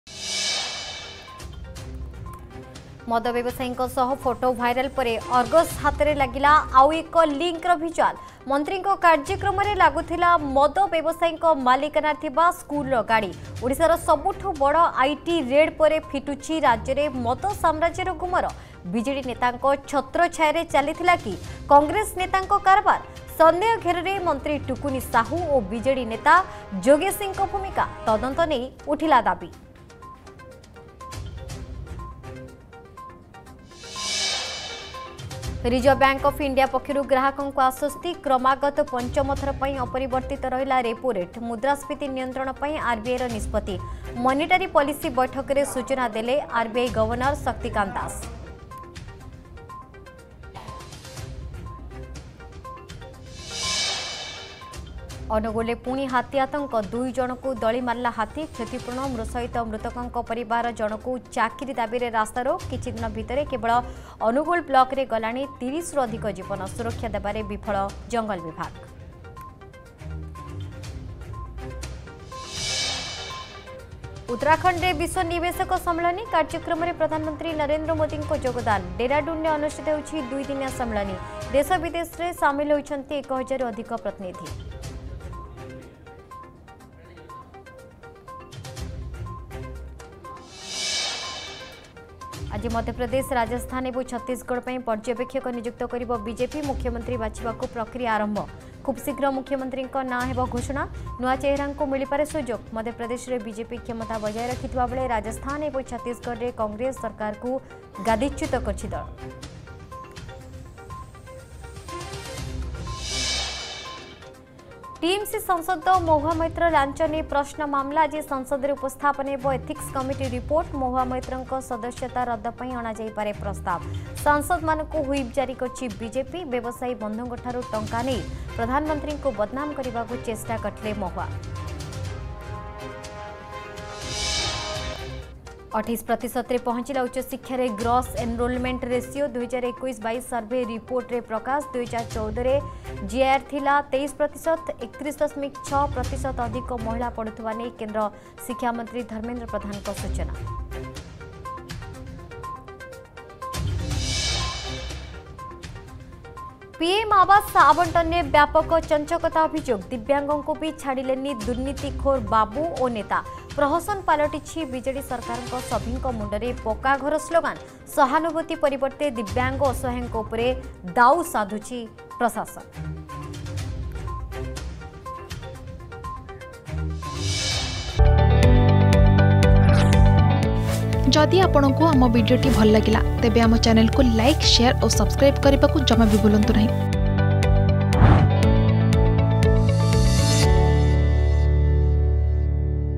Headlines @11Am | 8 Dec 2023 | Argus News
+ମନ୍ତ୍ରୀଙ୍କ କାର୍ଯ୍ୟକ୍ରମରେ ମଦ ବେପାରୀଙ୍କ ଗାଡ଼ି
+ମୁଖ୍ୟମନ୍ତ୍ରୀ ବାଛିବେ ପର୍ଯ୍ୟବେକ୍ଷକ
+ ଟିଏମସି ସାଂସଦ ମହୁଆ ମୈତ୍ର ଲାଞ୍ଚ ନେଇ ପ୍ରଶ୍ନ ମାମଲା